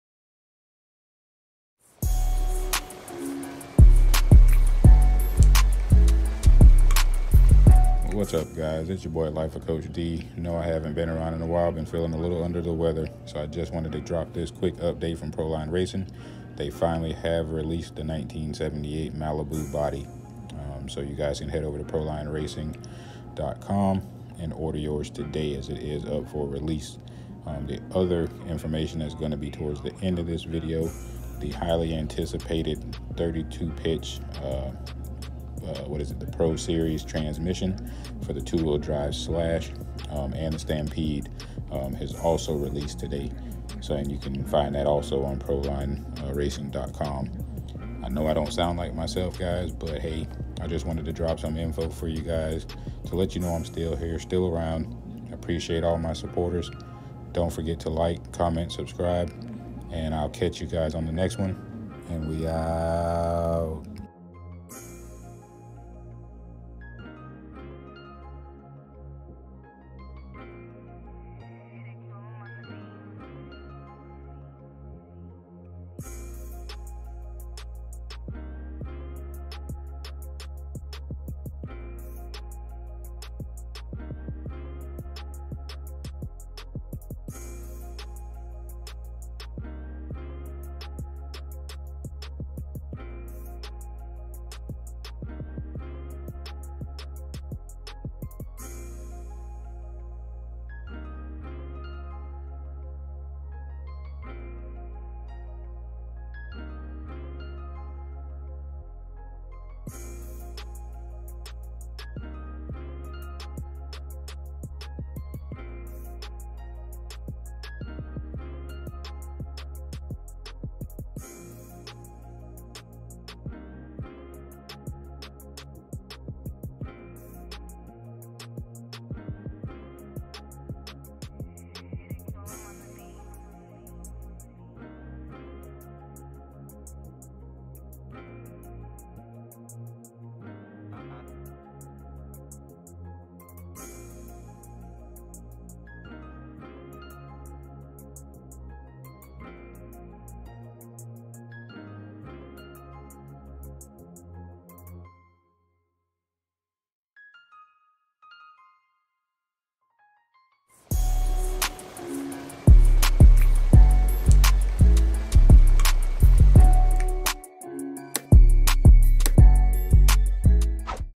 Proline Racing Malibu Body and 32p Transmission Install and Go
What's up Everyone! Welcome back to the channel. We will have Product Reviews, RC Bashing/Racing, Tutorials and Vlogs.

UD RC Website below for your rc needs...